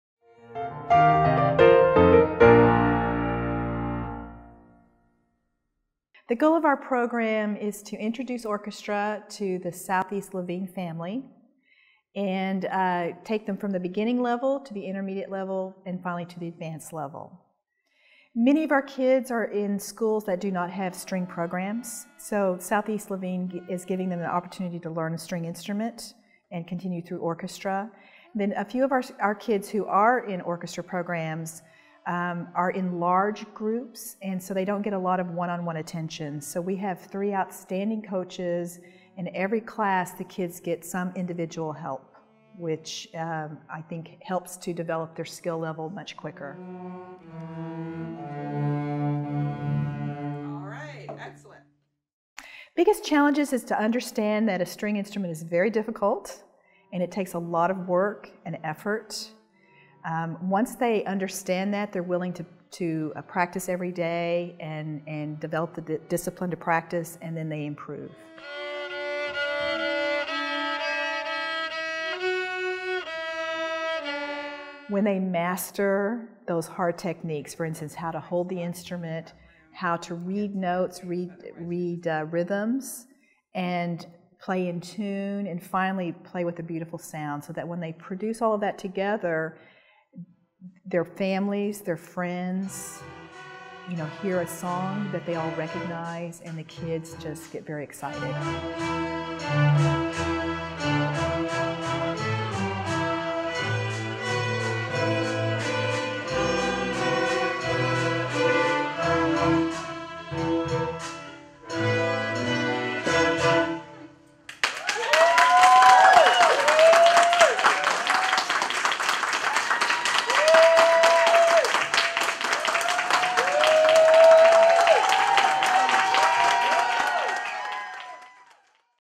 THEARC Beginner Orchestra
A look into our Beginner Orchestra program at THEARC in Southeast DC.